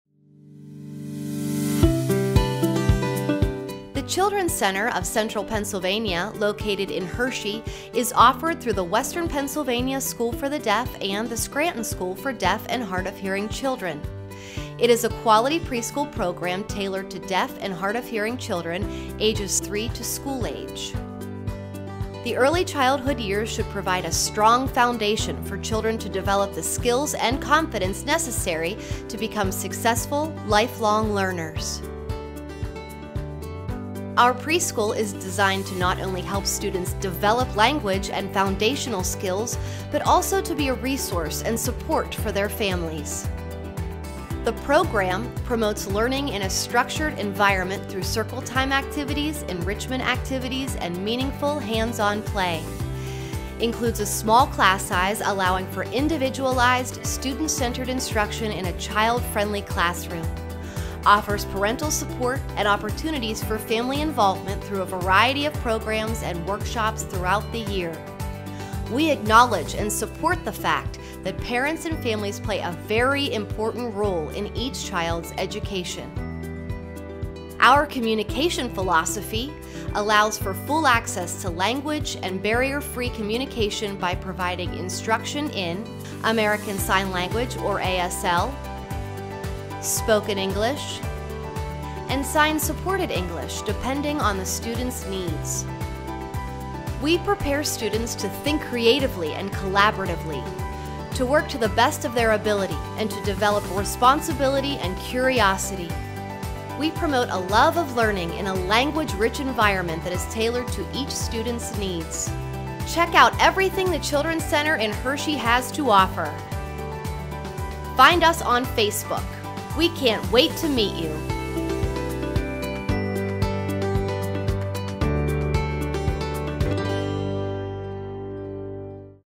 The Children's Center of Central Pennsylvania
The early childhood years should provide a strong foundation for children to develop the skills and confidence necessary to become succesful learners. The Children's Center of Central PA recognizes family as the key influence in a young child's life.  Our program is designed to your child develop the ncessary language, skills and knowledge to expand beyond home, but also to support you in your role as a parent of a child with a hearing loss.  We believe in a student-center approach to teaching and  learning; and prepare students to think creatively and collaboratively, to work to the best of their abilty, and to develop eresonsilbty and curiosity.